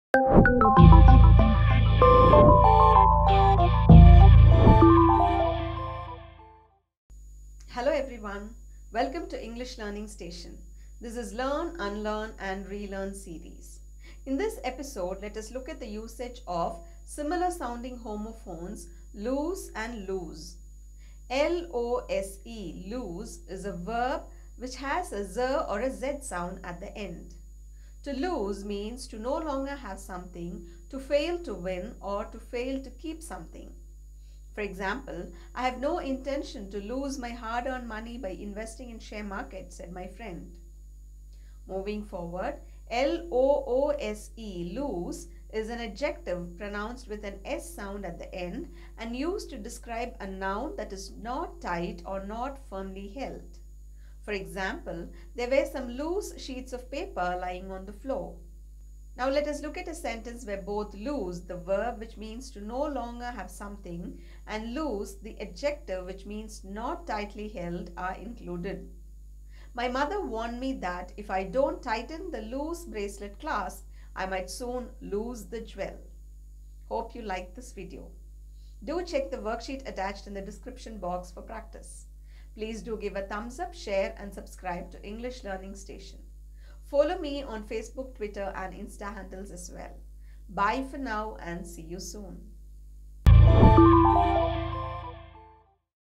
LoseVsLoose EnglishLearningStation LearnUnlearnRelearn
Usage of LOSE & LOOSE

L-O-S-E, Lose is a verb

To lose means to no longer have something, to fail to win or to fail to keep something.

For example
“I have no intention to lose my hard-earned money by investing in share market”, said my friend.

L-O-O-S-E, Loose is an adjective used to describe a noun that is not tight or not firmly held.

For example
There were some loose sheets of paper lying on the floor.

A sentence where both lose, the verb which means to no longer have something and loose, the adjective which means not tightly held, is given below:

My mother warned me that if I don’t tighten the loose bracelet clasp, I might soon lose the jewel.